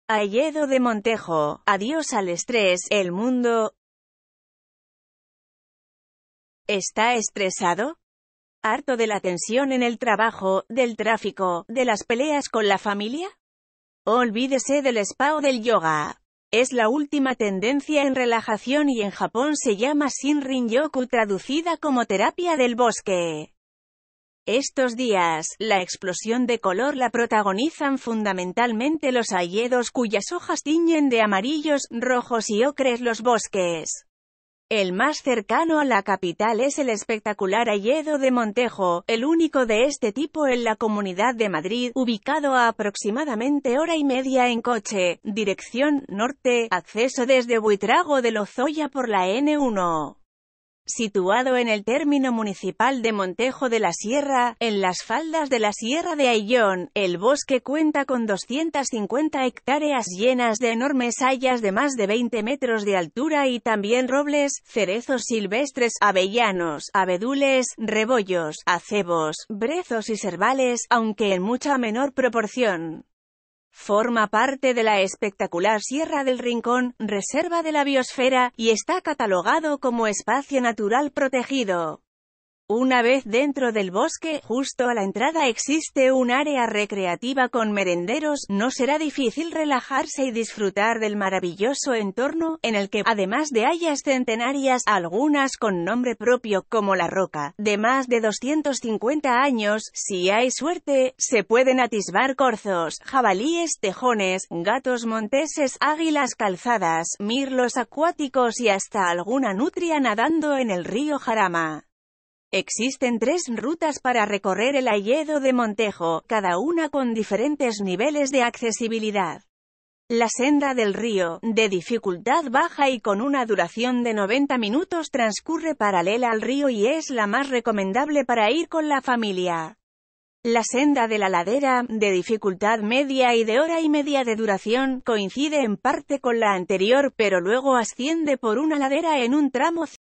Hayedo de montejo: adiós al estrés (elmundo)
¿está estresado? ¿harto de la tensión en el trabajo, del tráfico, de las peleas con la familia? olvídese del spa o del yoga. Es la última tendencia en relajación y en japón se llama shinrin-yoku (traducida como terapia del bosque). Estos días, la explosión de color la protagonizan fundamentalmente los hayedos, cuyas hojas tiñen de amarillos, rojos y ocres los bosques. El más cercano a la capital es el espectacular hayedo de montejo, el único de este tipo en la comunidad de madrid, ubicado a aproximadamente hora y media en coche, dirección norte (acceso desde buitrago de lozoya por la n-1). Situado en el término municipal de montejo de la sierra, en las faldas de la sierra de ayllón, el bosque cuenta con 250 hectáreas llenas de enormes hayas de más de 20 metros de altura y también robles, cerezos silvestres, avellanos, abedules, rebollos, acebos, brezos y serbales, aunque en mucha menor proporción. Forma parte de la espectacular sierra del rincón, reserva de la biosfera, y está catalogado como espacio natural protegido. Una vez dentro del bosque (justo a la entrada existe un área recreativa con merenderos), no será difícil relajarse y disfrutar del maravilloso entorno, en el que, además de hayas centenarias (algunas con nombre propio, como la roca, de más de 250 años), si hay suerte, se pueden atisbar corzos, jabalíes, tejones, gatos monteses, águilas calzadas, mirlos acuáticos y hasta alguna nutria nadando en el río jarama. Existen tres rutas para recorrer el hayedo de montejo, cada una con diferentes niveles de accesibilidad. La senda del río (de dificultad baja y con una duración de 90 minutos) transcurre paralela al río y es la más recomendable para ir con la familia. La senda de la ladera (de dificultad media y de hora y media de duración) coincide en parte con la anterior, pero luego asciende por una ladera en un tramo circular. La senda del mirador (de dificultad alta) lleva también un recorrido ascendente hasta alcanzar la ventana del hayedo o mirador, con una de las panorámicas más bonitas de la cuenca alta del jarama.